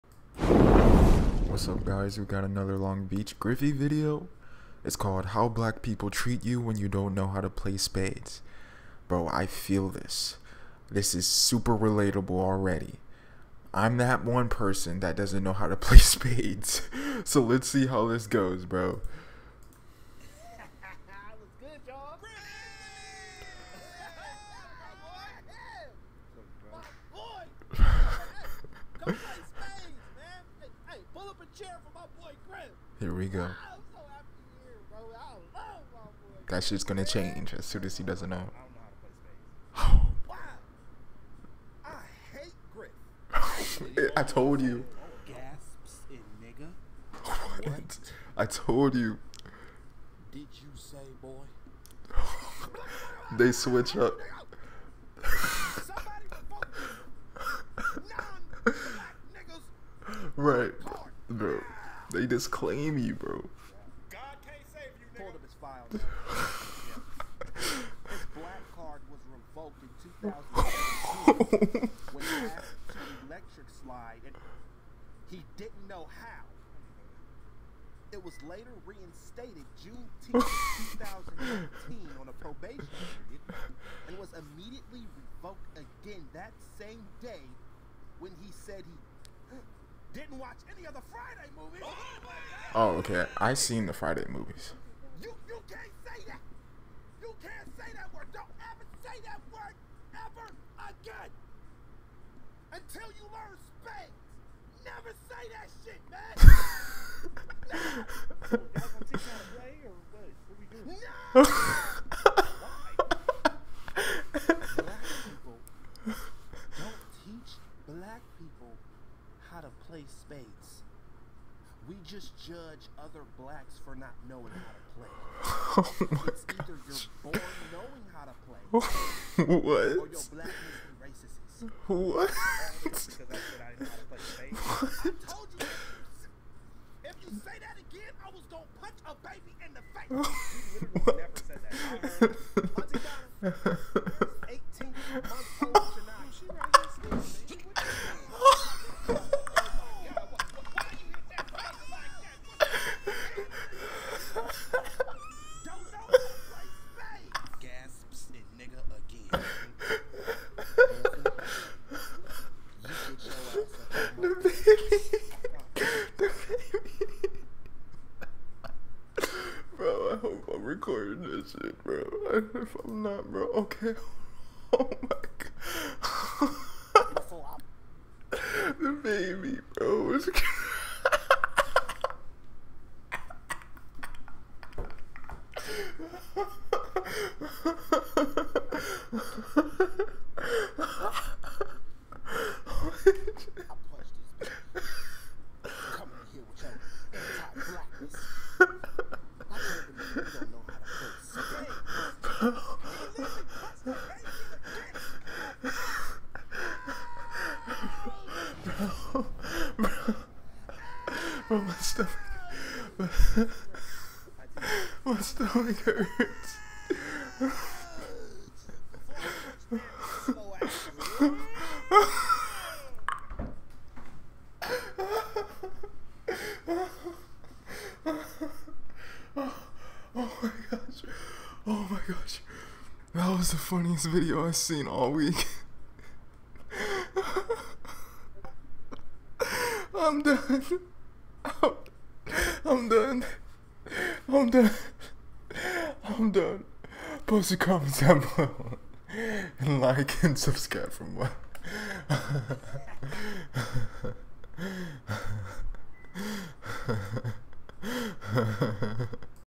How black people treat you when you don't know how to play Spades LONGBEACHGRIFFY REACTION LMAOOO!!!
bro. BRO I FELT THIS ONE!!...